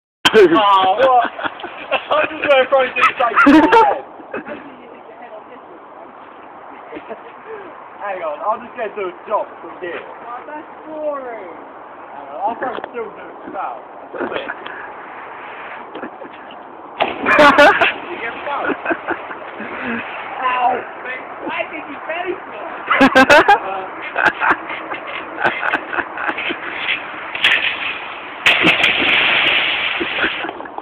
diving comp
2 of my m8s havin a comp of divin in a pool